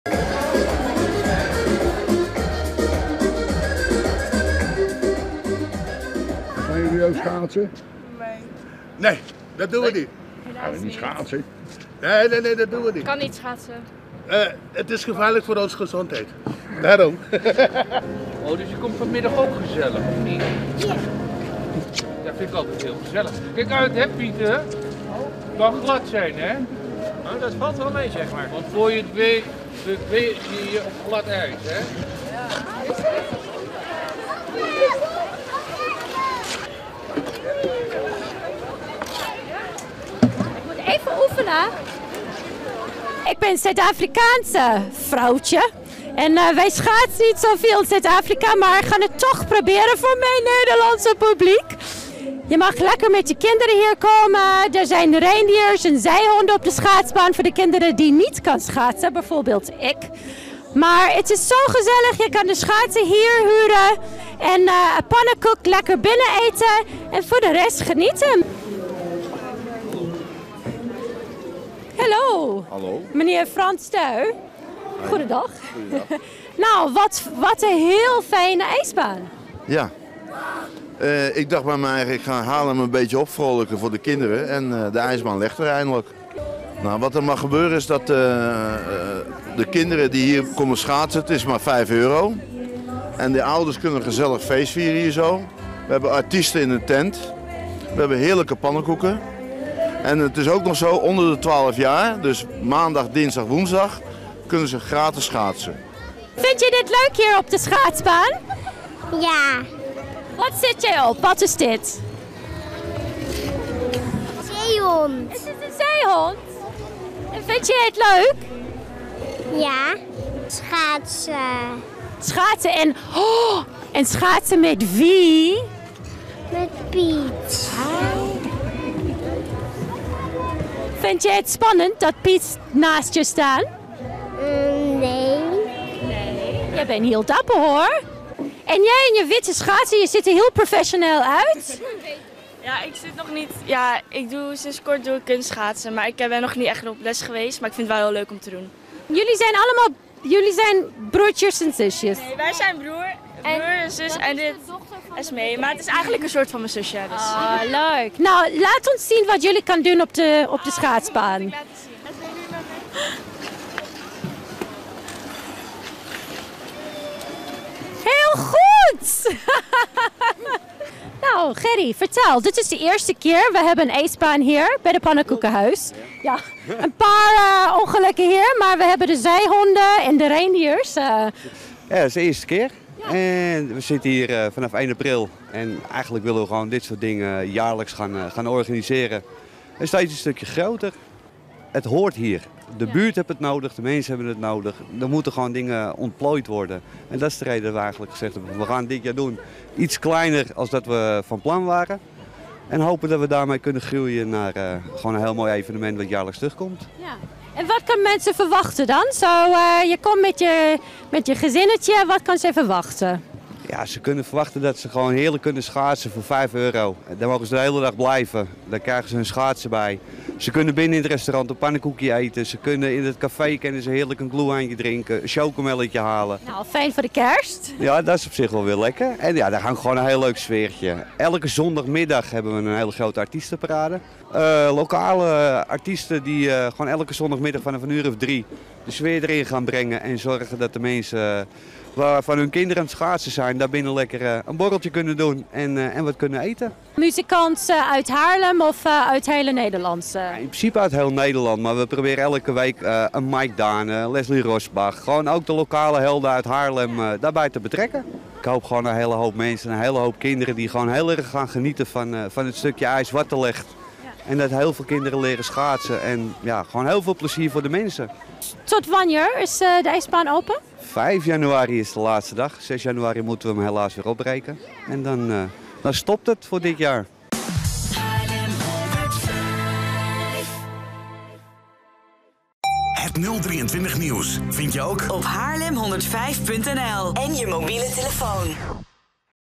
Voor het eerst schaatsen in het Reinaldapark | Haarlem105
Het heeft wat ambtelijke voeten in de aarde gehad maar eindelijk is het zo ver; er is een echte schaatsbaan in het Reinaldapark!